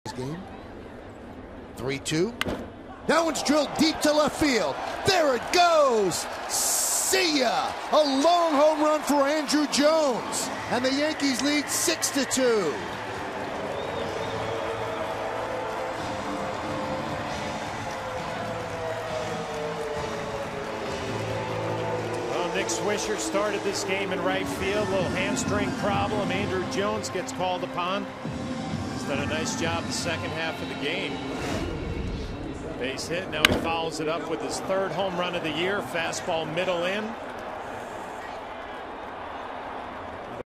2012/04/29 Jones' solo blast
DET@NYY: Jones launches a towering solo shot to left